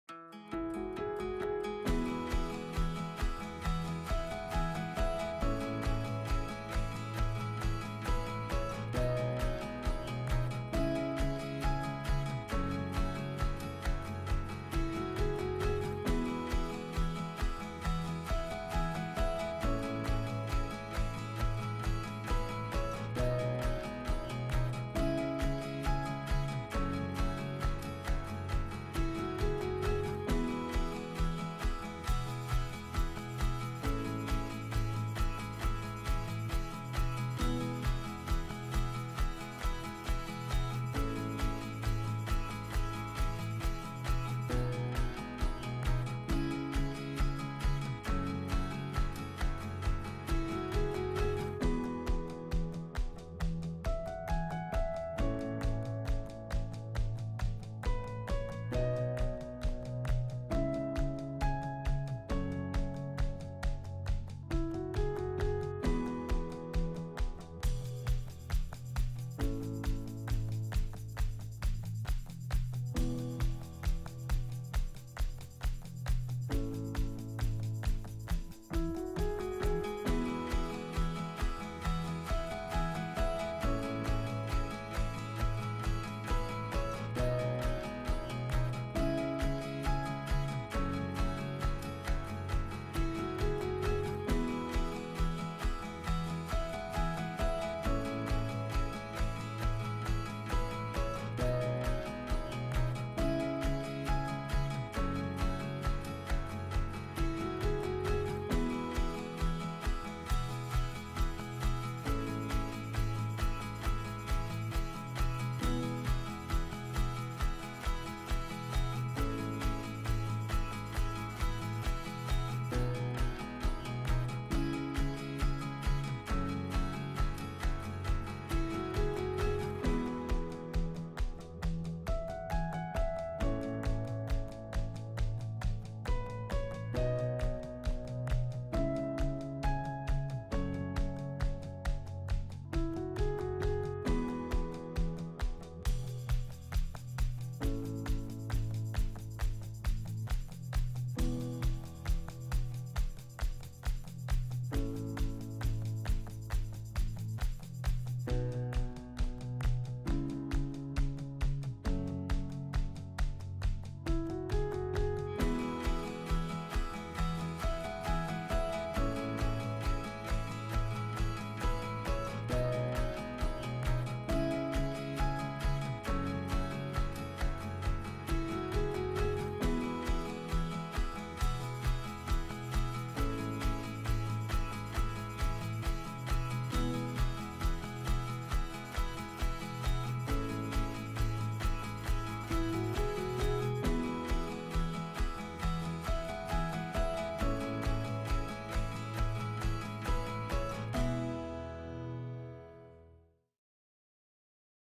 Unboxing Twinkle Pact Crystal Pet Shop and Crystal Restaurant Compact Playsets
We unboxed the Twinkle Pact Crystal Pet Shop and Restaurant Compact Playsets today.